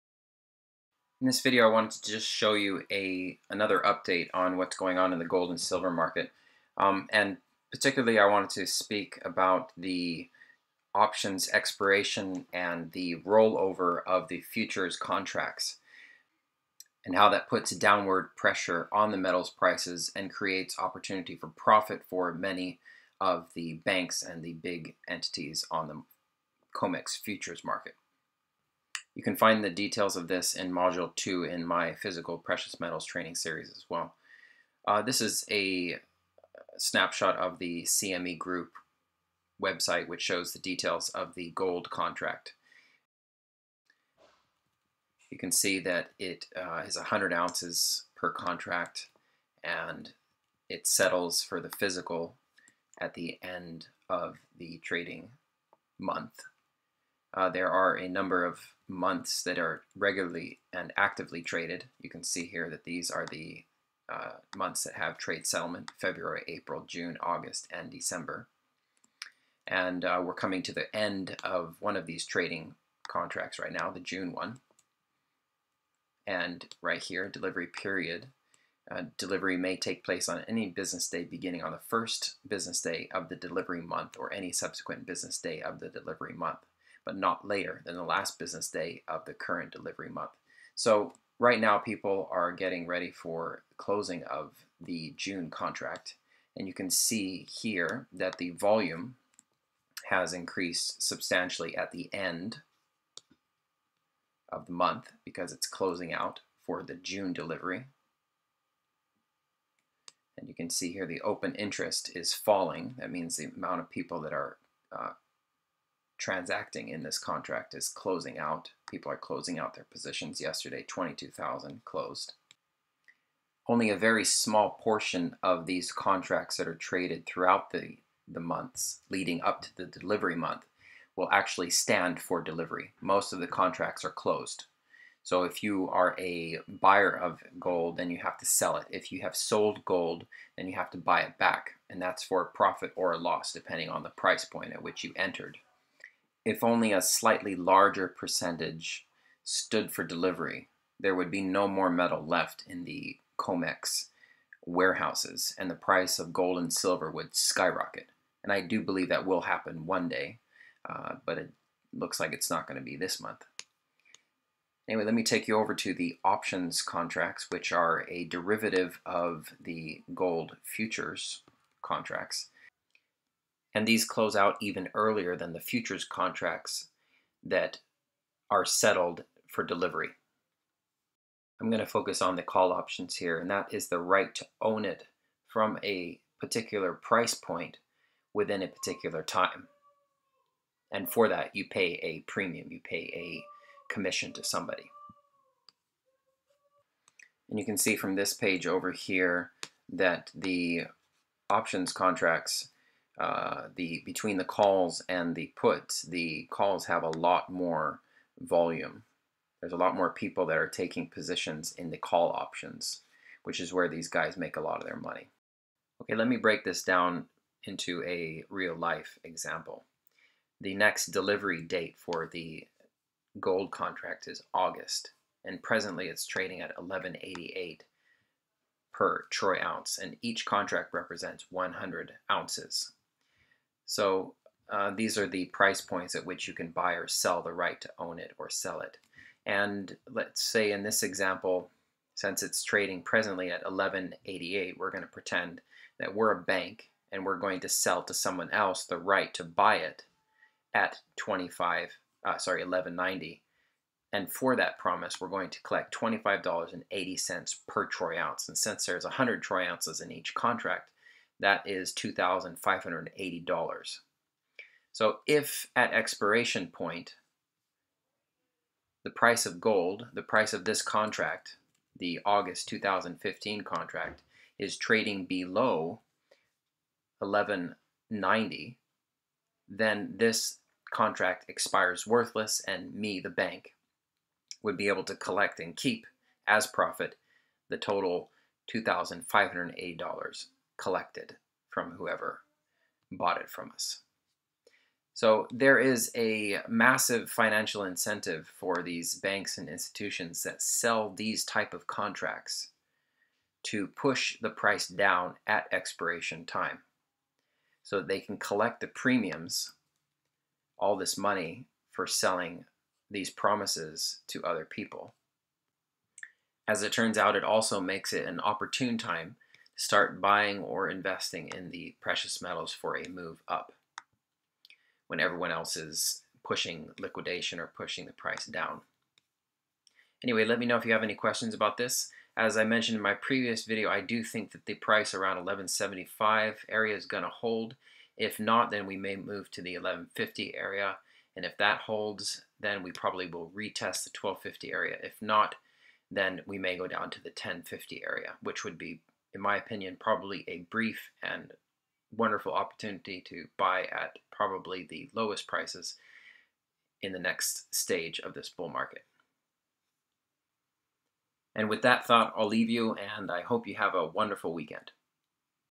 20150528update
Update on Gold Silver Expiration Dates, Roll Over, Futures contracts. Opportunity to purchase on price declines.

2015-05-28 Update some futures, options talk. --When you get handed lemons make lemonade.